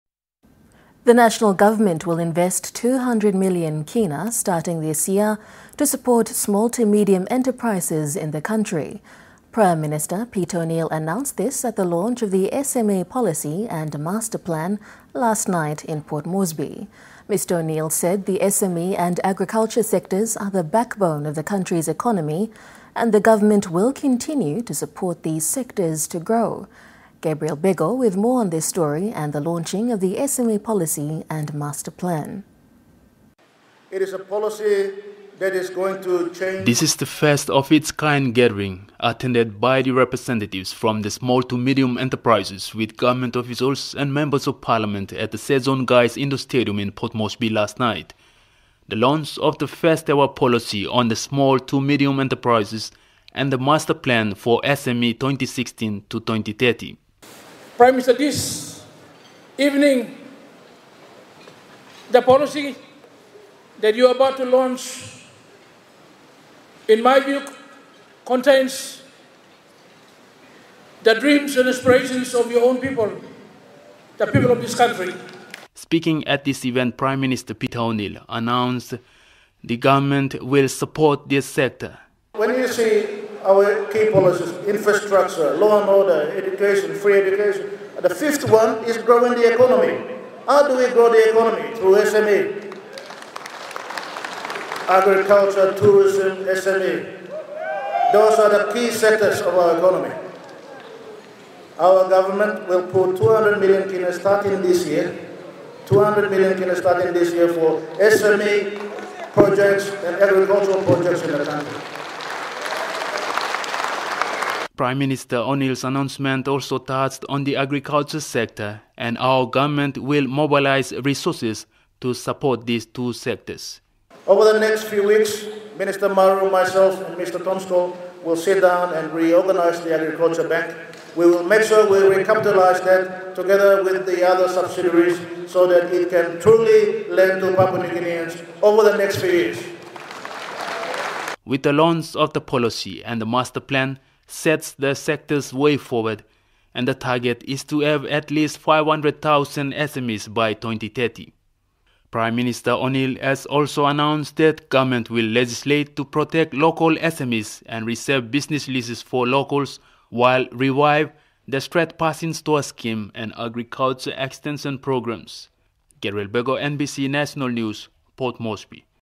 Government to Invest in SME and Agriculture_NBC News
The National Government will invest K200 million starting this year to support Small to Medium Enterprises in the country.